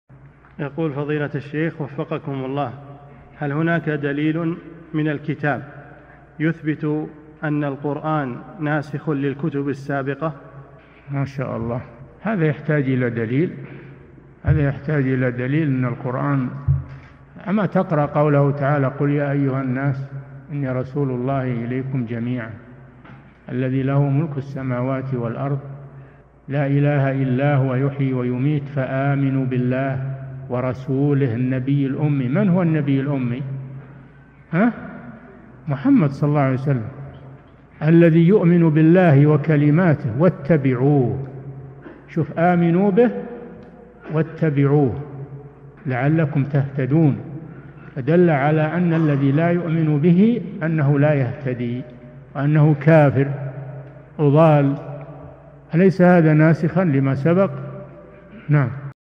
[1016 -1024] هل هناك دليل من الكتاب يثبت أن القرآن ناسخ للكتب السابقة؟ - الشيخ صالح الفوزان
-"[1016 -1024] السؤال: هل هناك دليل من الكتاب يثبت أن القرآن ناسخ للكتب السابقة؟
الجواب: ما شاء الله! هذا يحتاج إلى دليل؟! هذا يحتاج إلى دليل من القرآن؟ أما تقرأ قوله تعالى: ﴿قُلْ يَا أَيُّهَا النَّاسُ إِنِّي رَسُولُ اللهِ إِلَيْكُمْ جَمِيعًا الَّذِي لَهُ مُلْكُ السَّمَاوَاتِ وَالْأَرْضِ لَا إِلَهَ إِلَّا هُوَ يُحْيِي وَيُمِيتُ فَآمِنُوا بِاللهِ وَرَسُولِهِ النَّبِيِّ الْأُمِّيِّ﴾[الأعراف : 158] من هو النبي الأمي؟! محمد صلى الله عليه وسلم ﴿الَّذِي يُؤْمِنُ بِاللهِ وَكَلِمَاتِهِ وَاتَّبِعُوهُ﴾[الأعراف : 158] شف آمنوا به واتبعوه لعلكم تهتدون، فدل على أن الذي لا يؤمن به أنه لا يهتدي وأنه كافر وضال، أليس هذا ناسخًا لما سبق؟!
للاشتراك في قناة الشيخ صالح الفوزان
سؤال وجواب من شرح الفتوى الحموية ( 1024 ) مقطع

للاستفسار والإقتراحات والدعم
رئيس الفريق
لمشروع كبار العلماء